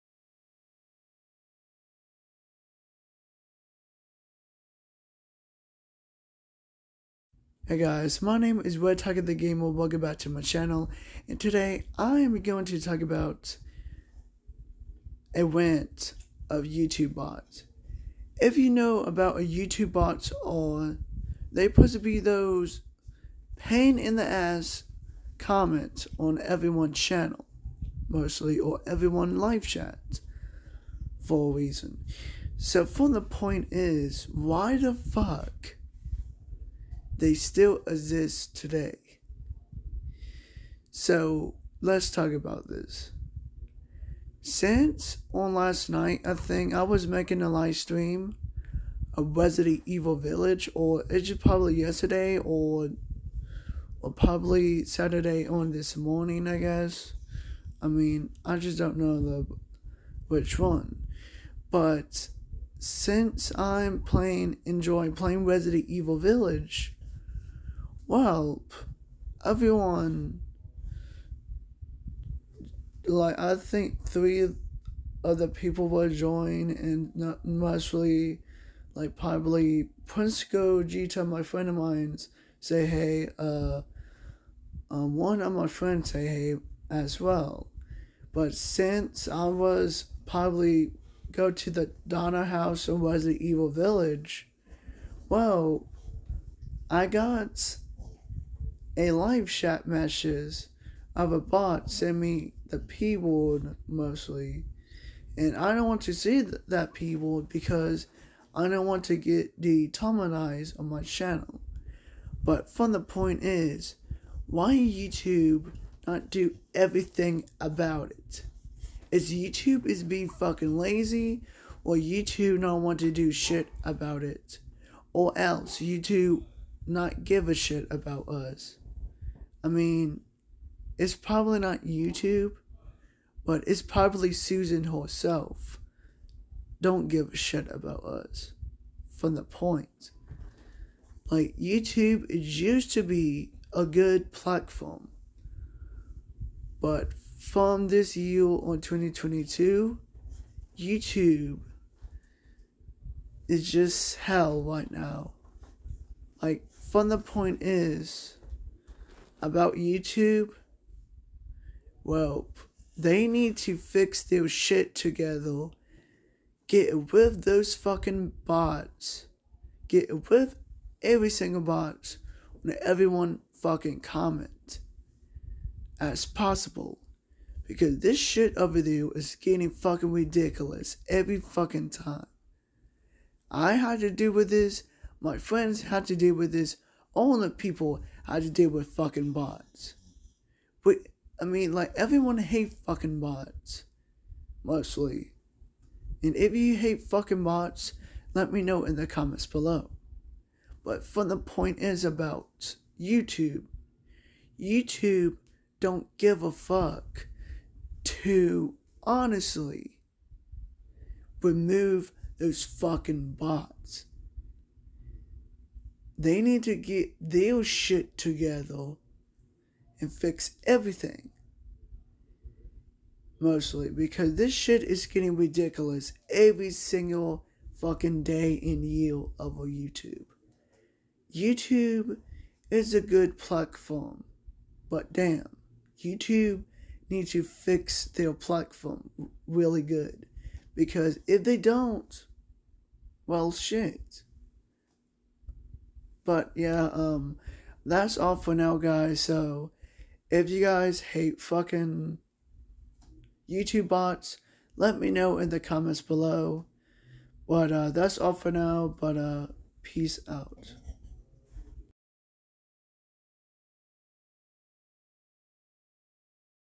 Youtube bots angry rant!
Today I'm going to rant about youtube bots.

Hope You Enjoyed The Video And Have A Great Day.